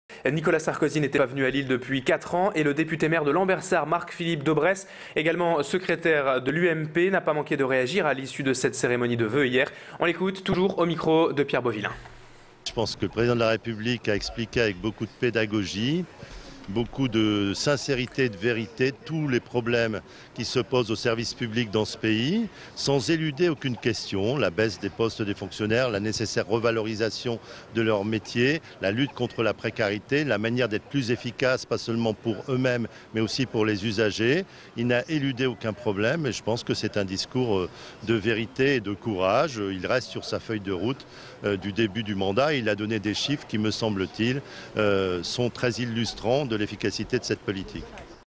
Sarkozy à Lille : Marc-Philippe Daubresse (UMP)
Nicolas Sarkozy était à Lille pour présenter ses voeux aux fonctionnaires. Marc-Philippe Daubresse,  député maire UMP de Lambersart, revient sur son intervention

Sites webs/blogs, copiez cette vidéo avec le bouton "Intégrer" !